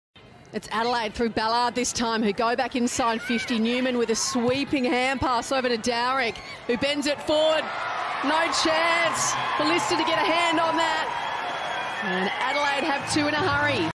AFLW R5: Dowrick delivers on her left.
Crows AFLW forward Mckenzie Dowrick kick truly on her left.